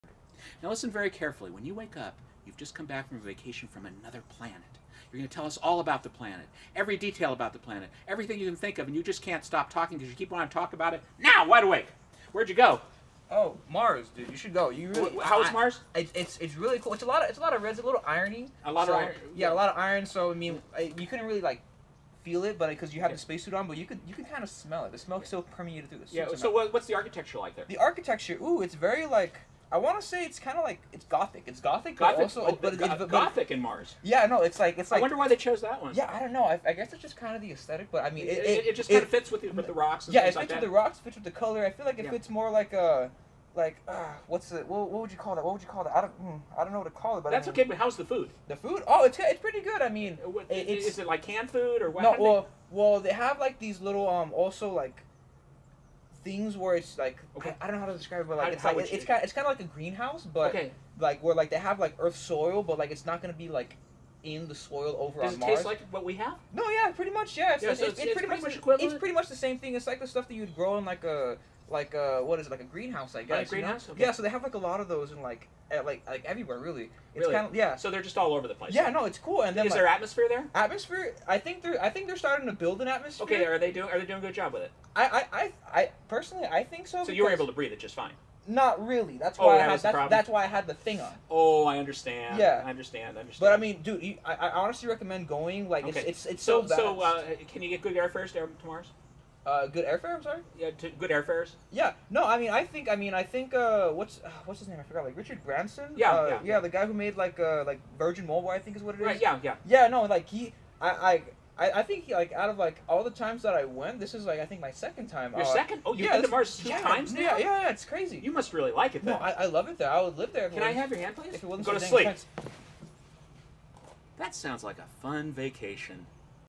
I Hypnotized Him to Think He Went to the Planet Mars and He Tells Us All About It
All aboard for a voyage into outer space in this hypnosis video! He has all the detail you would ever want to know about his trip to Mars, even if it is only in his own mind! This individual hypnosis video is a type of street hypnosis rather than a hypnosis show, and is very funny!